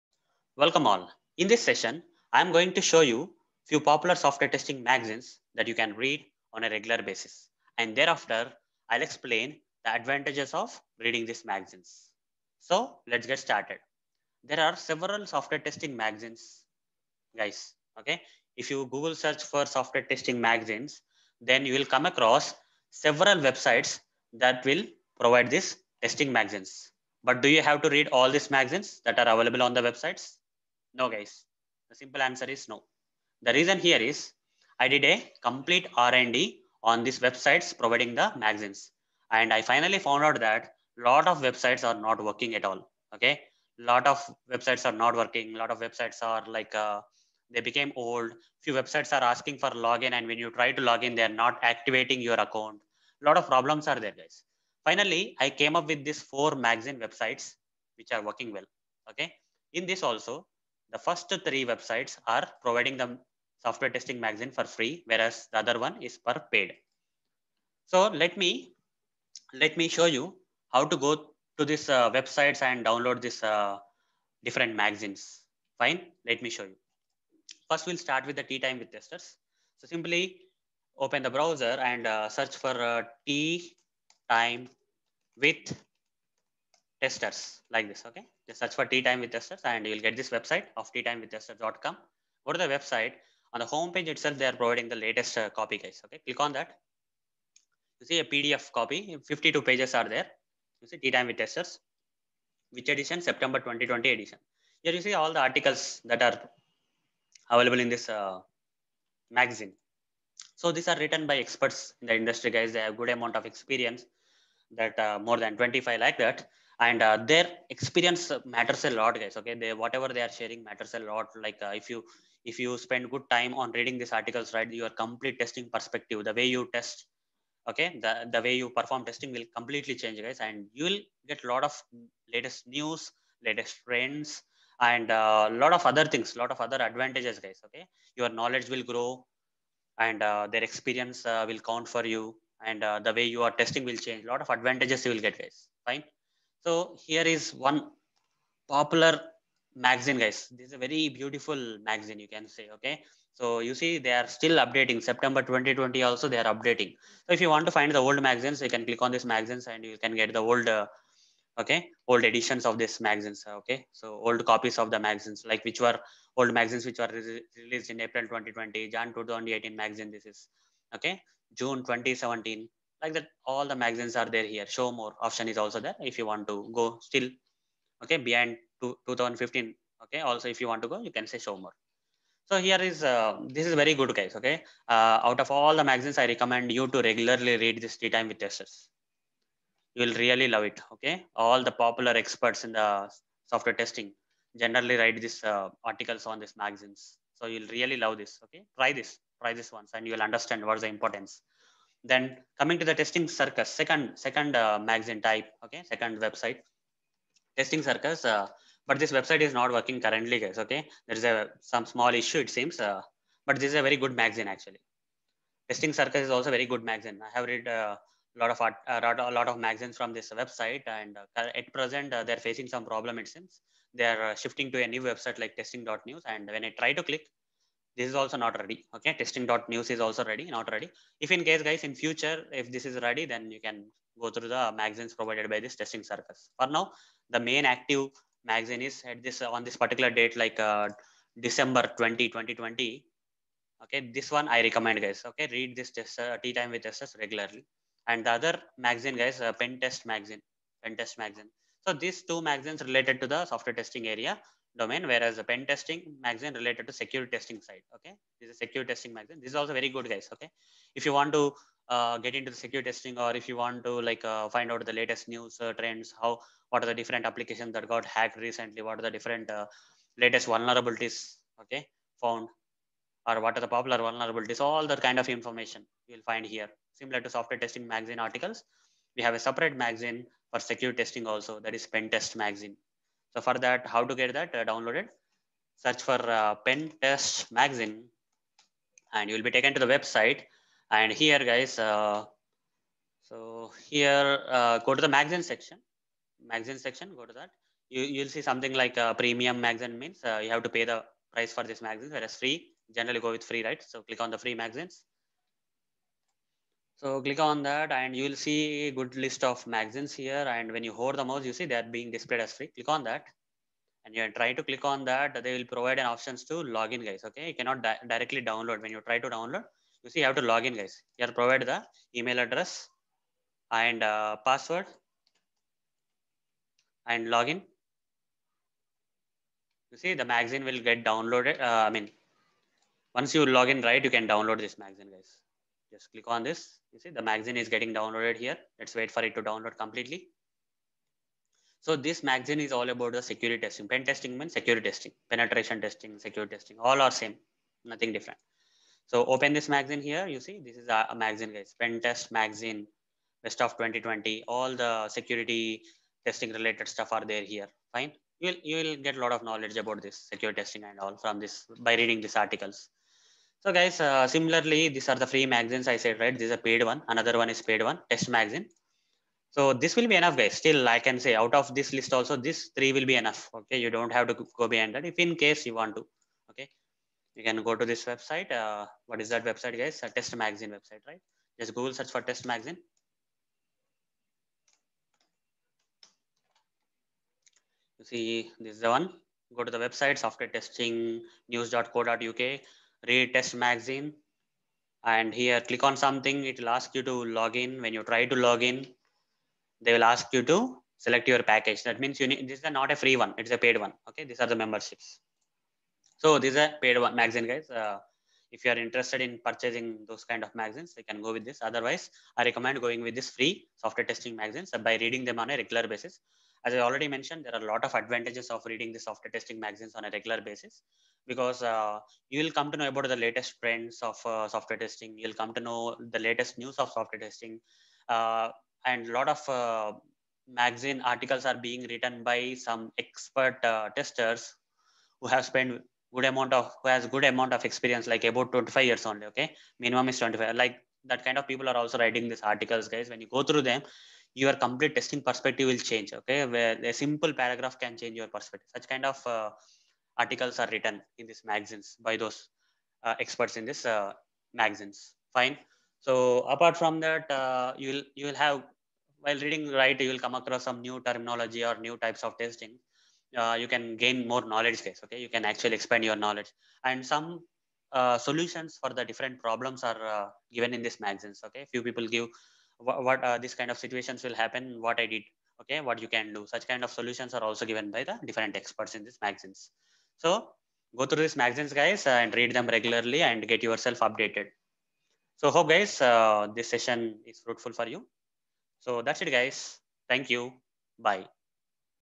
Read different Software Testing Magazines - Popular ones listed and shown
In this video, I have shown few popular Software Testing Magazines that you can read on a regular basis and also explained the advantages of reading these Magazines.